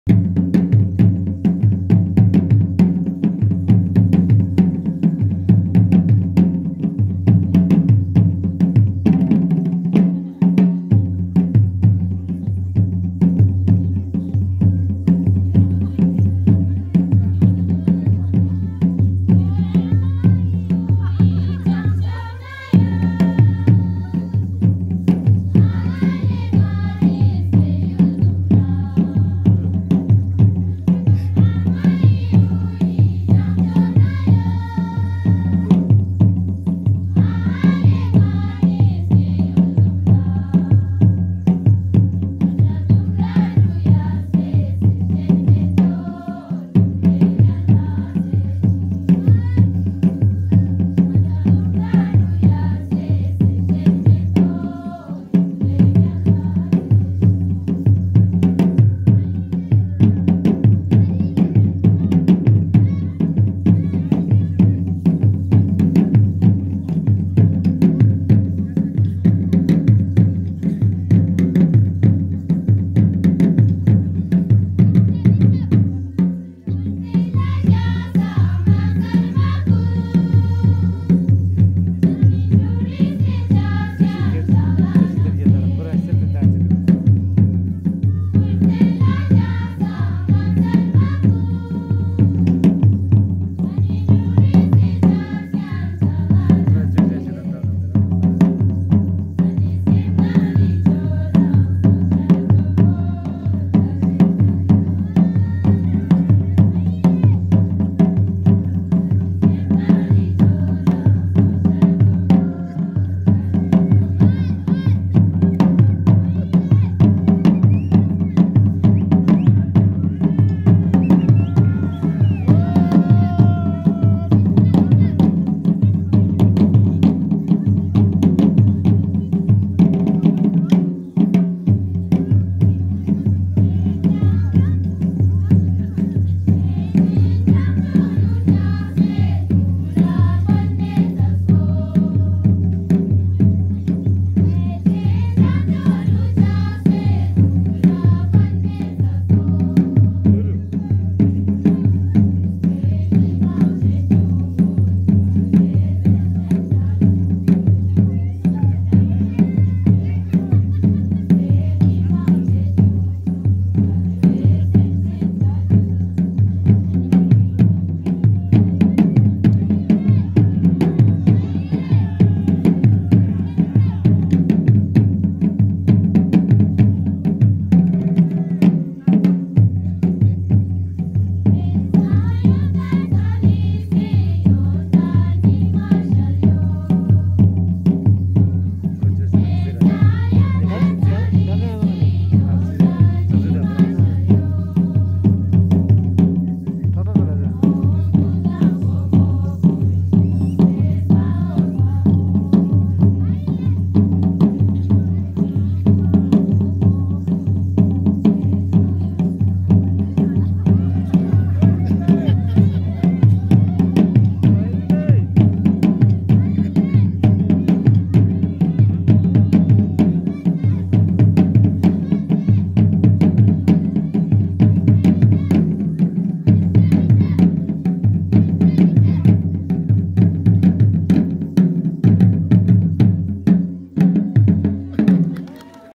Dance Led By YDA President Nubra kaga Tashi Dargyas ly..
Dance Led By YDA President Nubra kaga Tashi Dargyas ly 31 December 2021...
Welcome to my chanel in Drukpa jigwanG.
Do take care of yourself and your family.